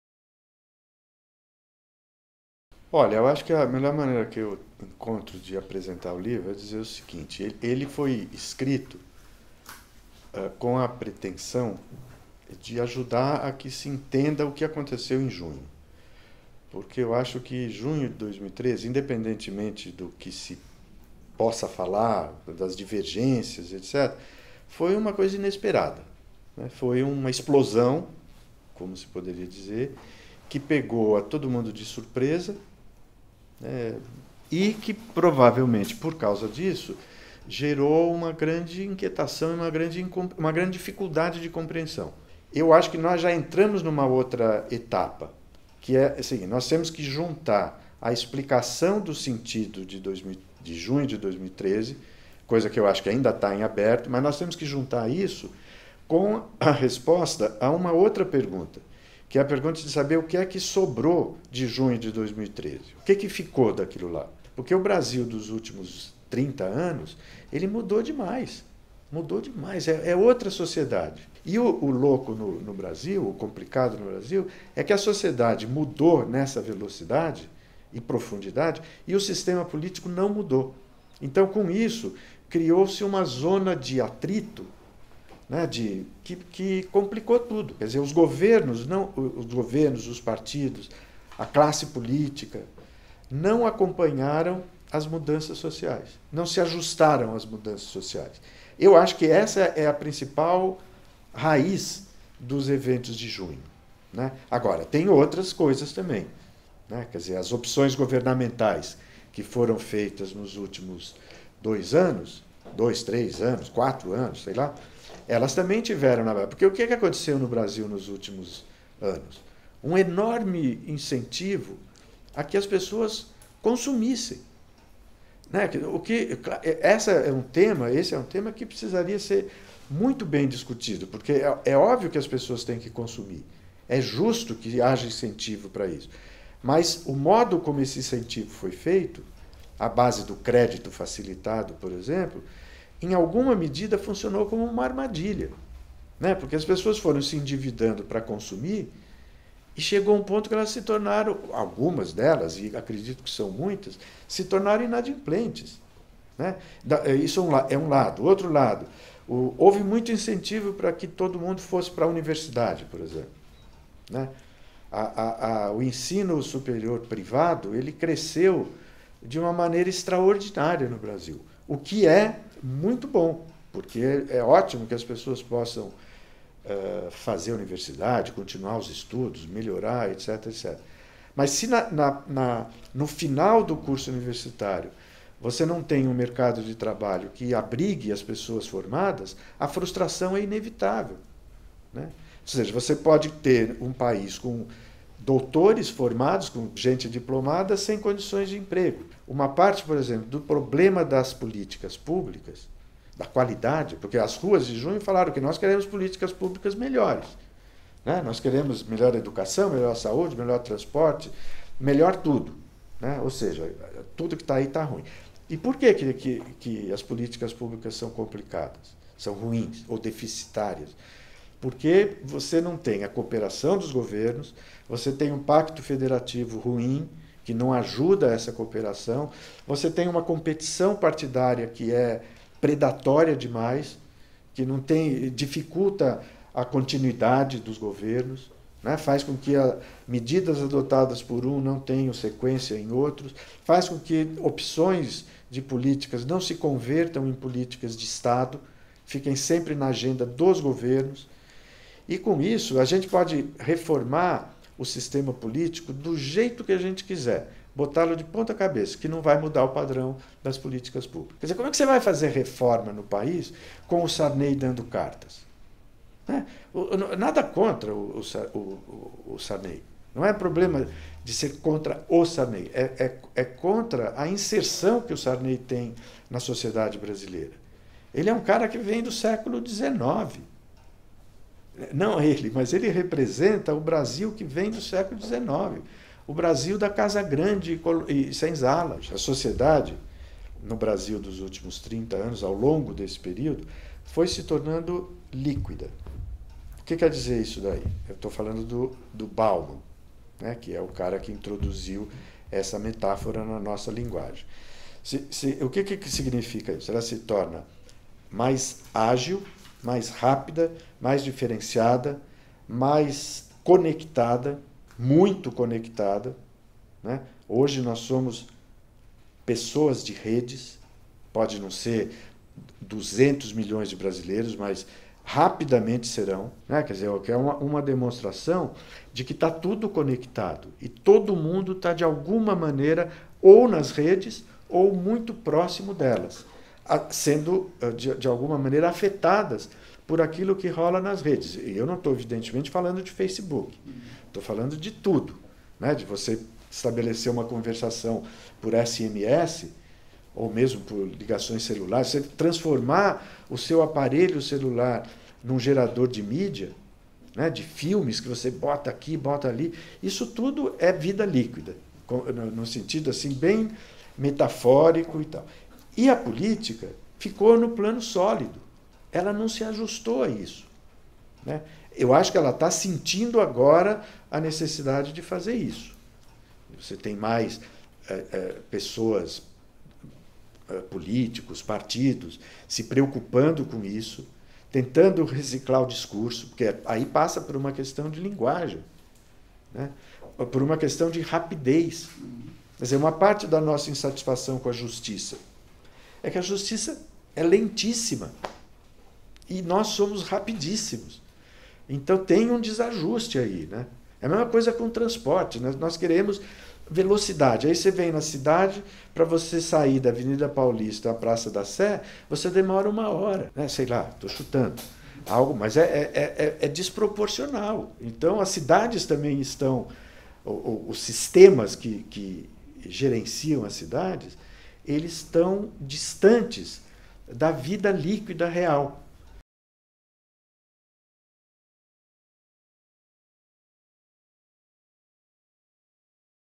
A política não acompanha a velocidade das mudanças sociais
Em novo livro, cientista político Marco Aurélio Nogueira, da Unesp, analisa as origens e as lições das manifestações pelo Brasil